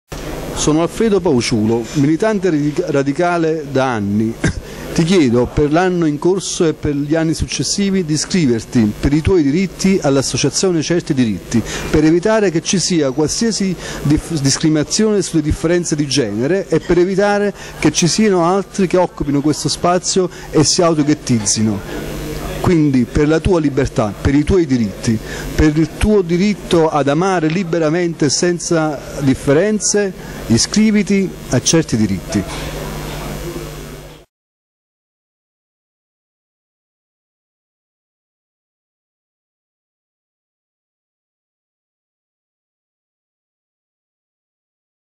Alfredo Pauciulo per Certi Diritti
Alfredo Pauciulo, del Comitato Nazionale di Radicali Italiani, è testimonial della campagna iscrizioni per il 2012 della nostra Associazione. Ascoltalo in questo breve video cosa ha da dirti sull'importanza di iscriversi per dare forza alle iniziative che promuoviamo con l'Associazione Radicale Certi Diritti.